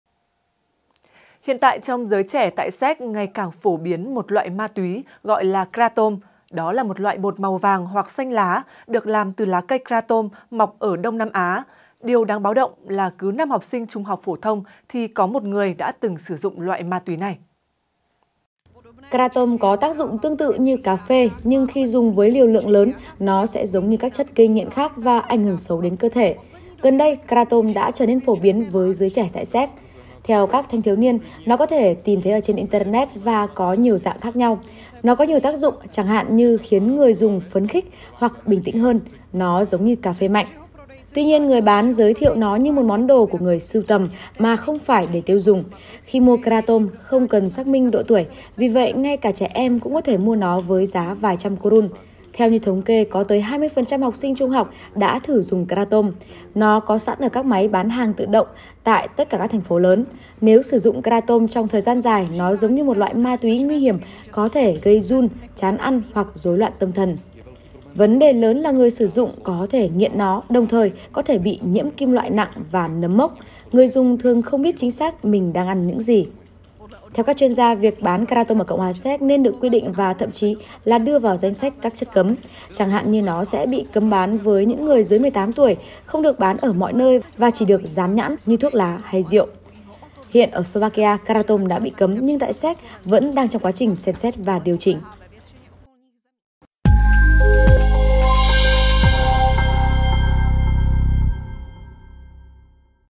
Kratom ngày càng phổ biến trong giới trẻ tại Séc | Tamdamedia
Hiện tại trong giới trẻ tại Séc ngày càng phổ biến một loại ma túy gọi là kratom. Đó là một loại bột màu vàng hoặc xanh lá được làm từ lá cây kratom mọc ở Đông Nam Á. Điều đáng báo động là cứ 5 học sinh trung học phổ thông thì có một người đã từng sử dụng loại ma túy này.